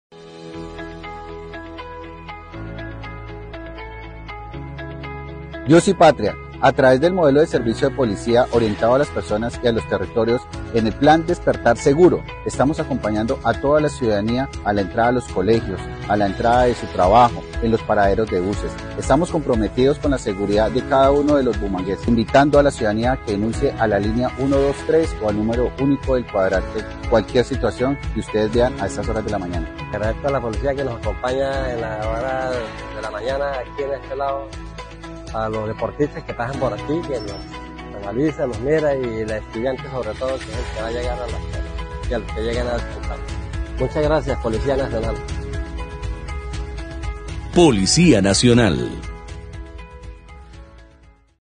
Plan despertar seguro de la Policia de Bucaramanga
Comprometidos con la Seguridad con cada uno de los Bumangueses, el mayor Richard Guzman comandante de la Estacion Sur de la policia metropolitana de bucaramanga , acompaña desde temprano a los Estudiantes, a los Deportistas y trabajadores con presencia policial y patrullajes en los alrededores de colegios, escenarios deportivos y paraderos de buses.

La ciudadania muestra su gratitud, con las acciones ordenandas por el Coronel Hnery Yesid Belos Cubides, comandante de la Mebuc